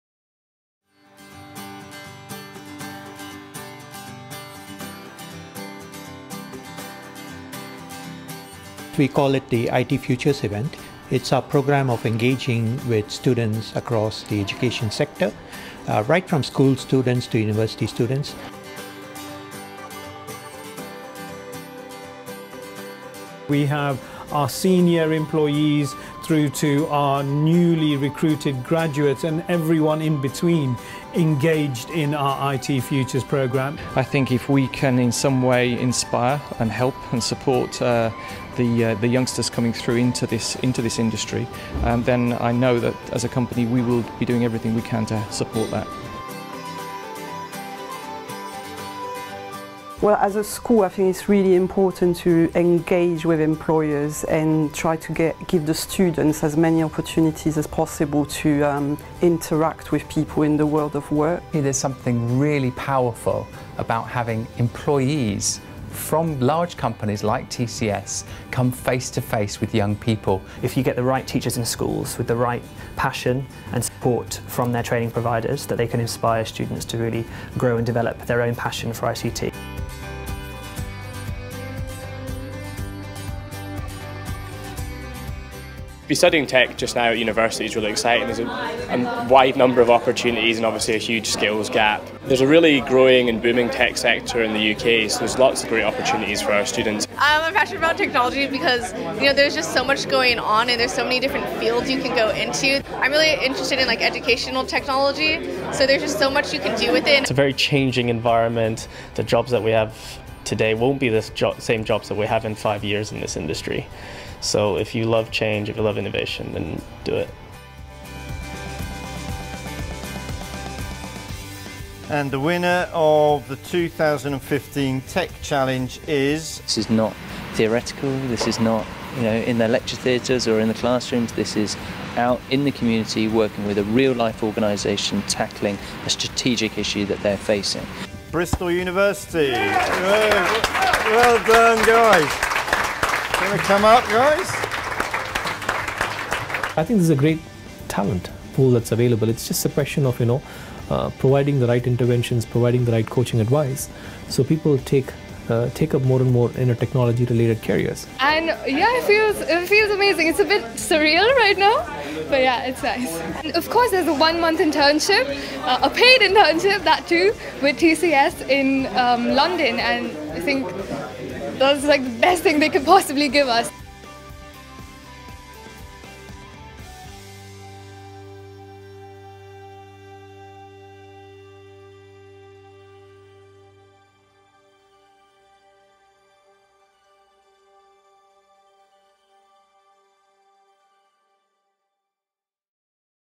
TCS IT Futures Program, UK
The IT Futures program works across numerous secondary schools and universities in the UK and Ireland to reconnect young people who have an innate enthusiasm for technology. It aims to show students that they can help to create the digital worlds they inhabit.